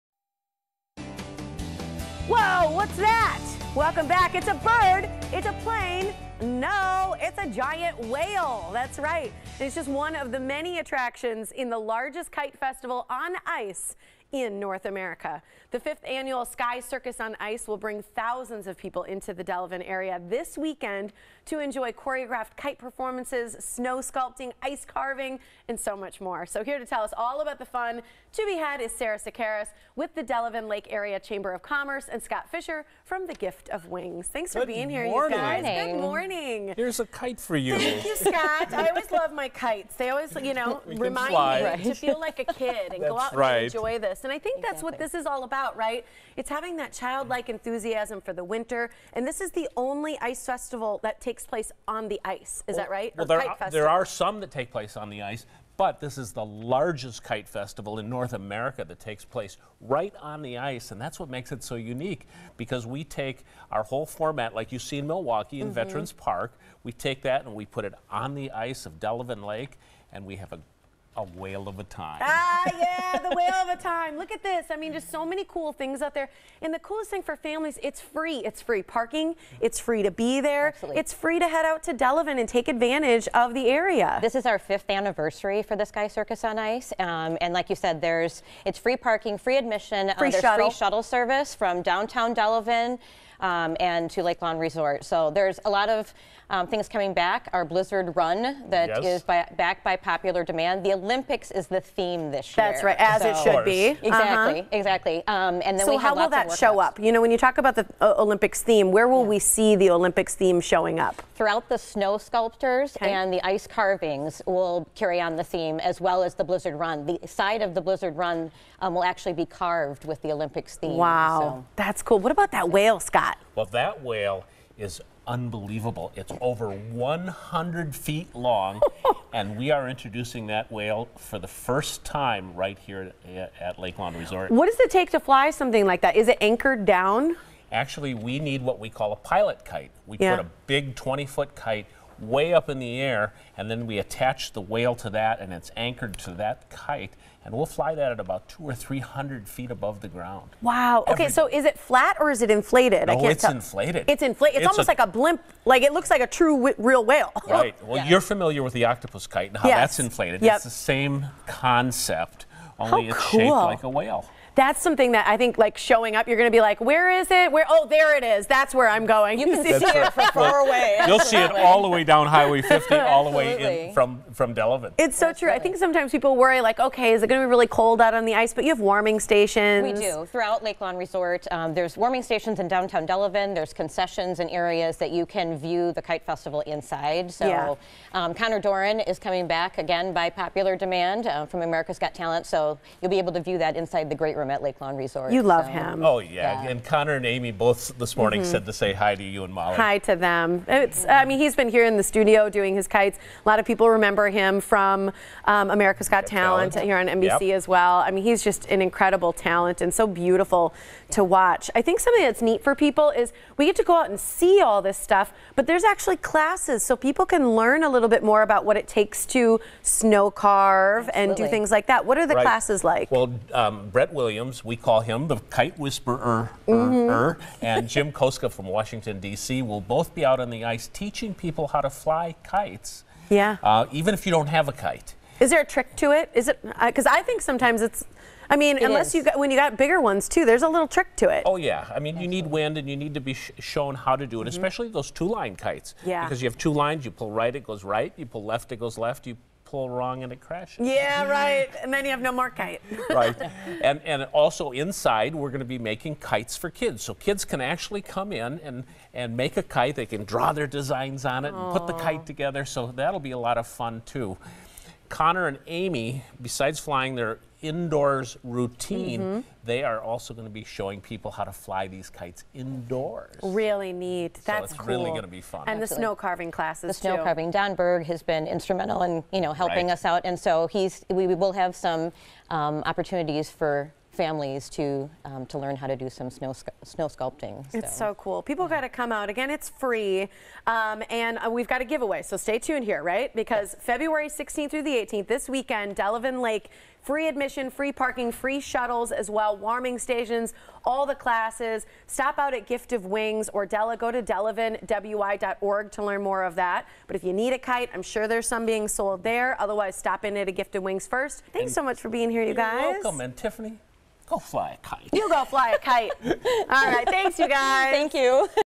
A One-of-a-Kind Winter Festival
The Sky Circus on Ice on Delavan Lake is the largest Kite Festival on Ice in North America and is now in its 5th consecutive year! There are incredible choreographed kite flying displays, ice sculptures, a snow slide, and so much more! Joining us with all the details are Scott Fisher from Gift of Wings and Sara Sekeres from the Delavan-Delavan Lake Area Chamber of Commerce.  Don't miss the 5th Annual Sky Circus on Ice this weekend, February 16 through February 18. Admission to this one-of-a-kind event is FREE!